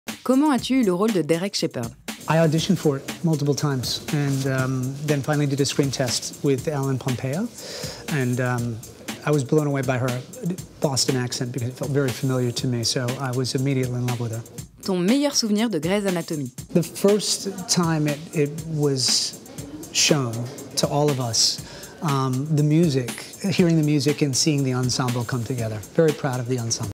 Patrick Dempsey remembers the first time he met Ellen Pompeo
Patrick was in Cannes to receive an Excellence Award for 30 years of his career in films and TV on 14th of October, 2019. During one of the interviews for Biinge, when questioned how he got the role as Derek Shepherd, Patrick brought up how he felt the first time he met his former co-star, Ellen Pompeo.